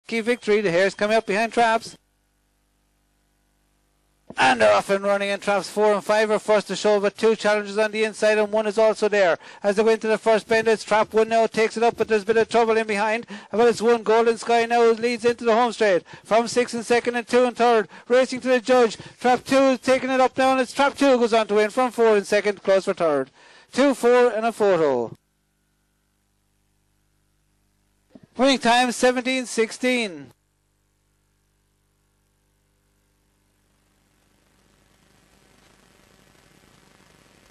IGB - The Retired Greyhounds Trust Sprint 06/10/2019 Race 6 - Clonmel
Race: The Retired Greyhounds Trust Sprint
Track Name: Clonmel
Date: 06/10/2019
Hour: 20:45

Results

1st    2     COOLBEG SADIE       5/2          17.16
2nd    4     DINNYS SOLO         2/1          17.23
3rd    1     GOLDEN SKY          7/4F         17.25
4th    6     KEY VICTORY         2/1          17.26
5th    5     RIVER WYLDE         2/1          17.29
6th    3     OBAIR NA MBAN       5/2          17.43

|CML_2019_10_06_06